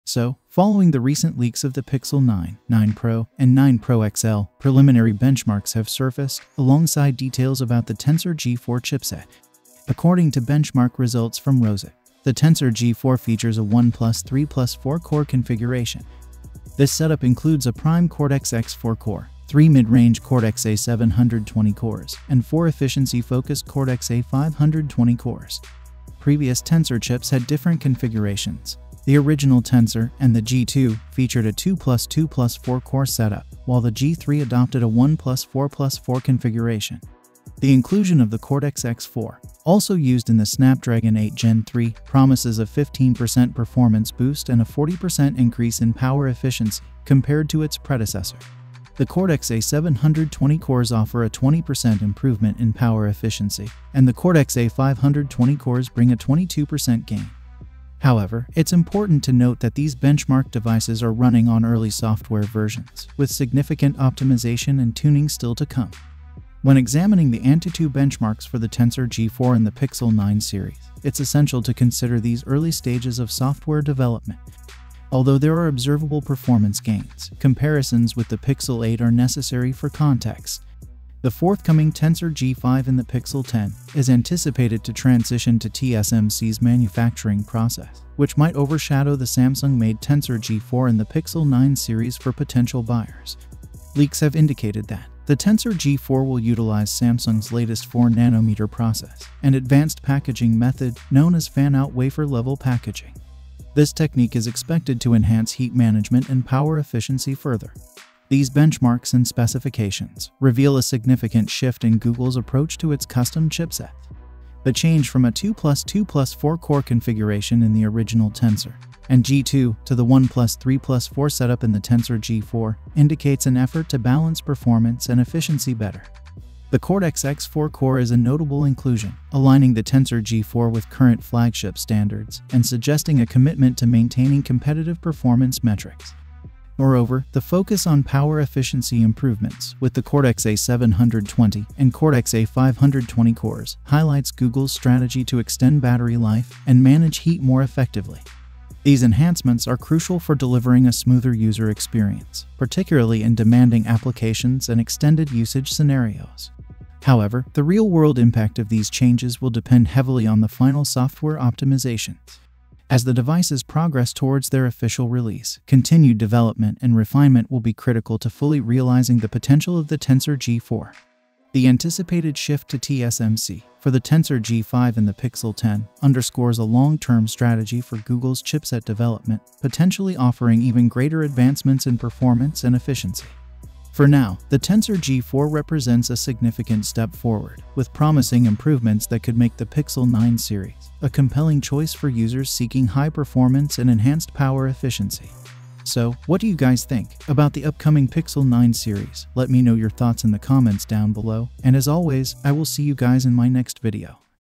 Google Pixel 9 Pro - NEW CHIPSET!
Following the recent leaks of the Pixel 9, 9 Pro, and 9 Pro XL, preliminary benchmarks have surfaced alongside details about the Tensor G4 chipset. According to benchmark results from Rozetked, the Tensor G4 features a 1+3+4 core configuration. This setup includes a prime Cortex-X4 core, three mid-range Cortex-A720 cores, and four efficiency-focused Cortex-A520 cores.